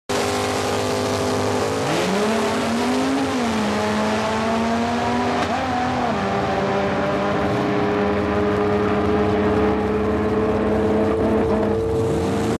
Big H Racing Ford XW GT at WSID Launching
Harry - big h xw at test n tune 25/07/10 with an awesome reaction time .002